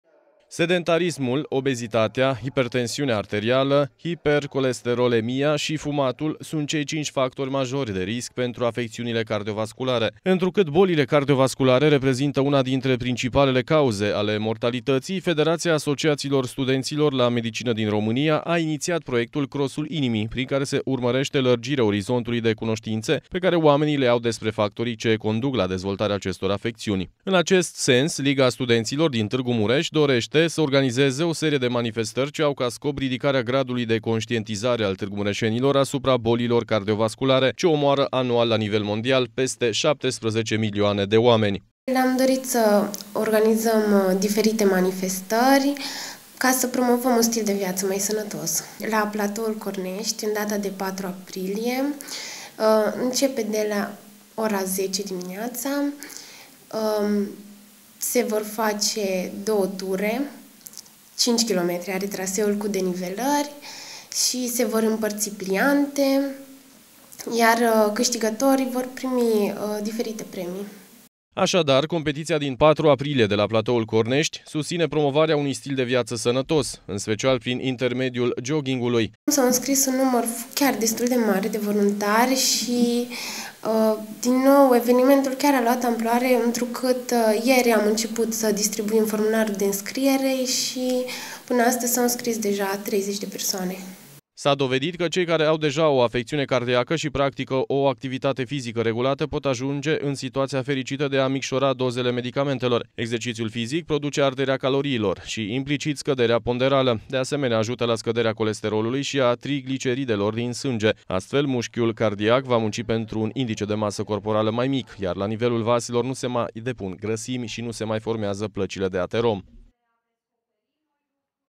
CROSUL INIMII (26.03.2015)
Sedentarismul este unul dintre cei cinci factori majori de risc pentru afecțiunile cardiovasculare. Începerea practicării treptate a unei activități fizice reduce considerabil șansele de apariție a unui eveniment coronarian acut și a accidentelor vasculare cerebrale. În acest sens Liga Studenților din Tîrgu Mureș demarează un nou proiect, ”Crosul inimii” .